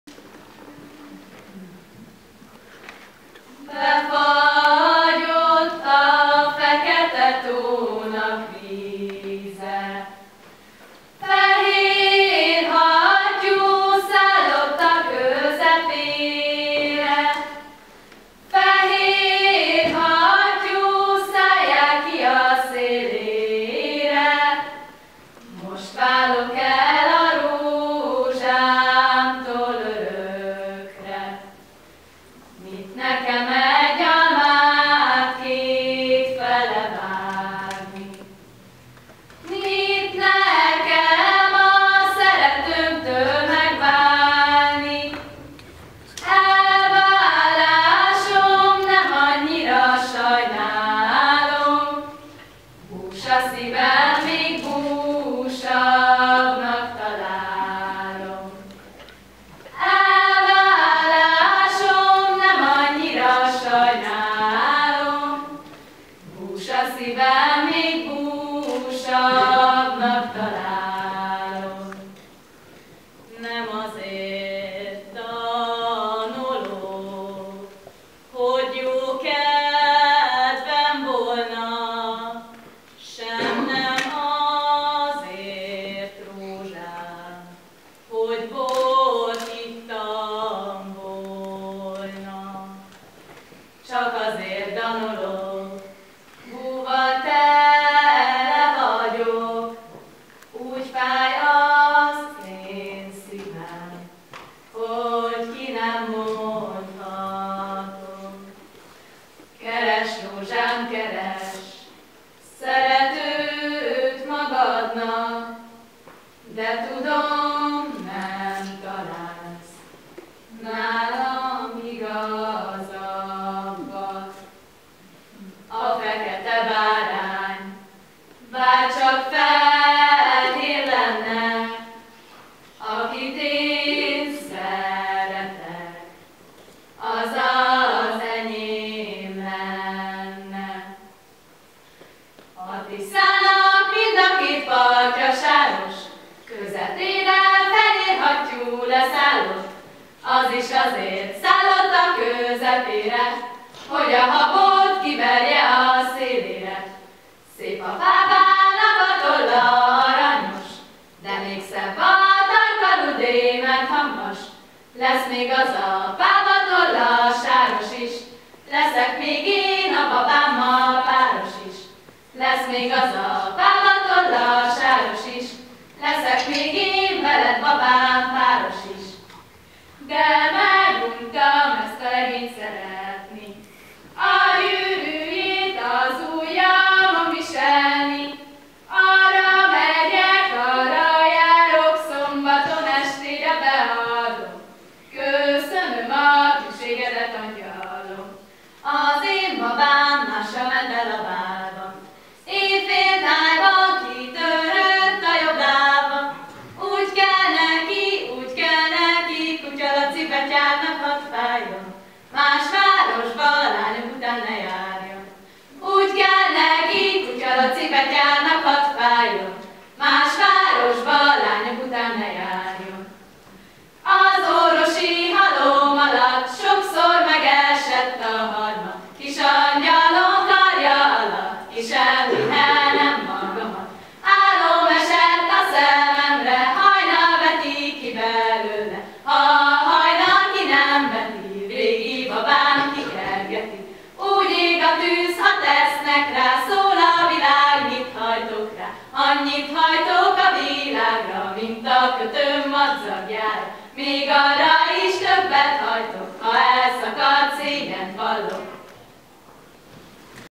Kisgöncöl - Bihari népdalok, Művészeti szemle Gála 2015
A Hajnalcsillag néptáncegyüttes Kosgöncöl szekciójától hallhatunk népdalokat.